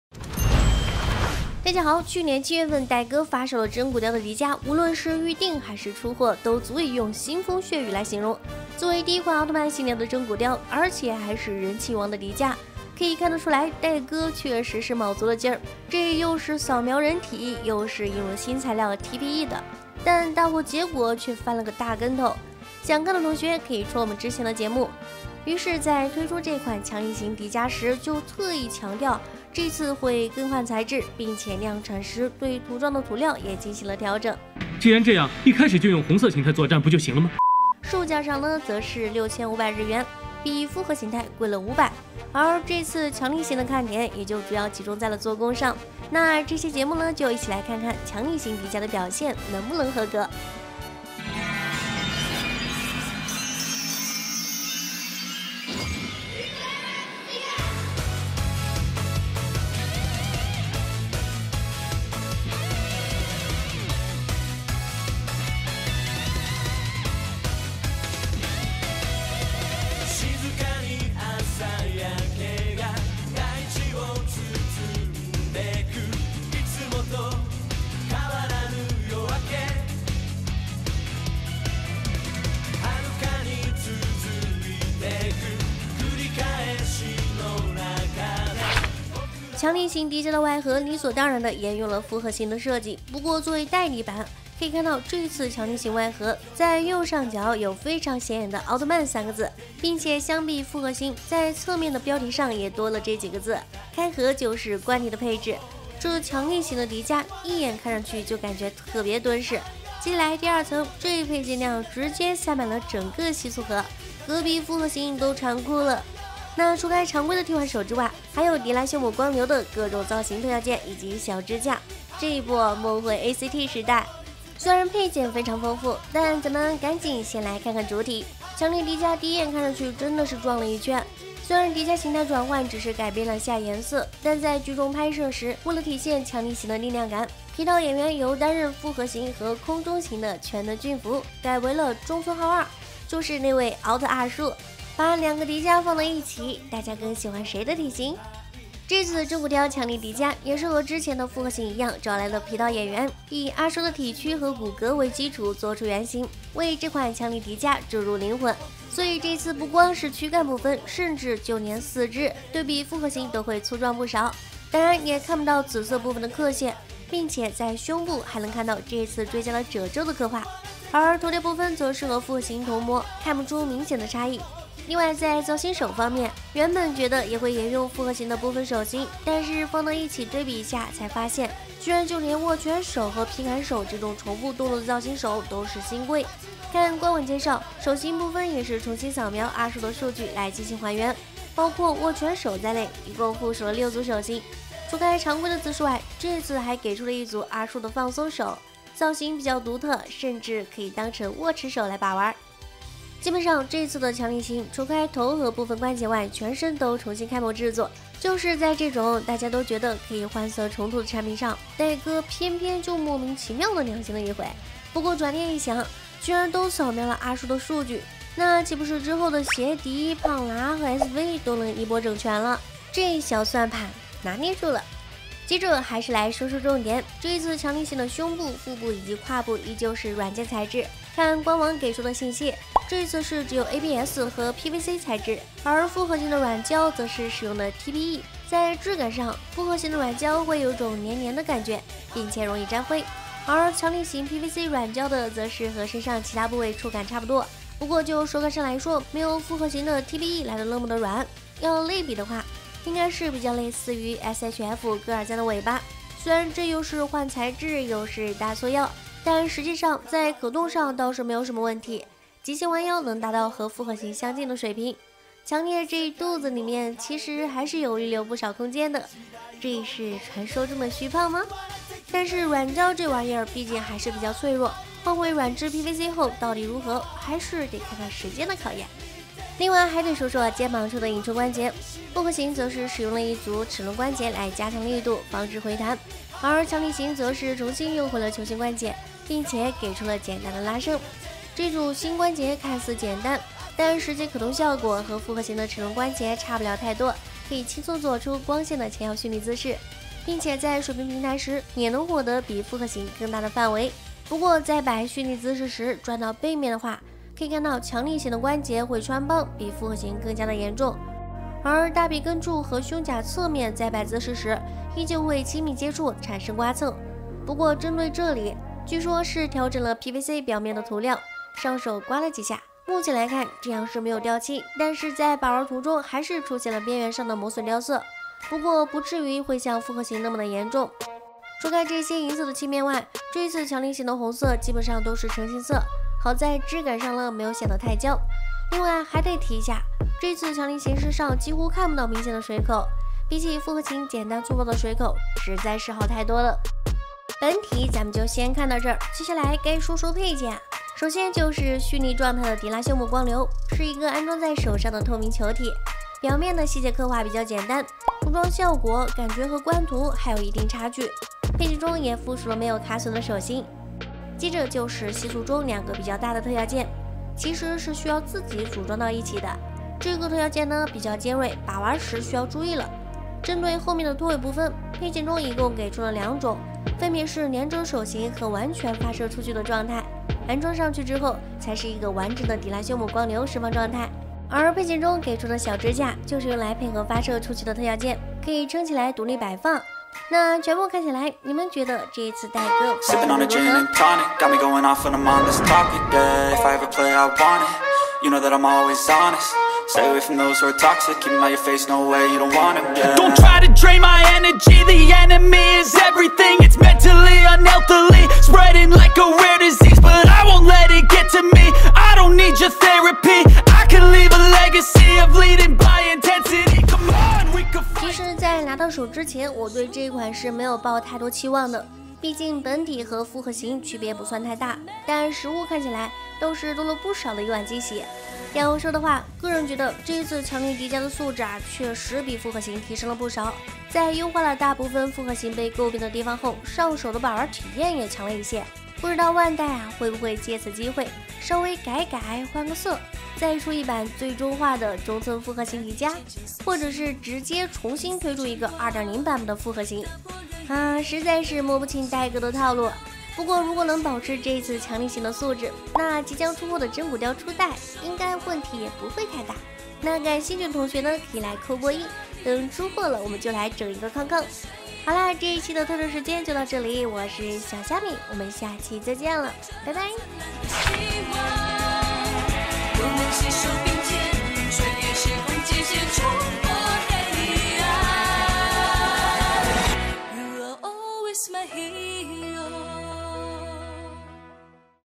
變胖了也變強了！ ~萬代真骨雕刻強力型迪迦介紹！ 【蝦米大模王】
去年7月的真骨雕迪迦直接掀起一陣腥風血雨，結果大貨素質只能說是代哥玩脫啦，各種掉漆翻車，所以在強力型迪迦裡直接改用了其他材質，那這期就來看看這次換了材質後代哥還會不會翻車！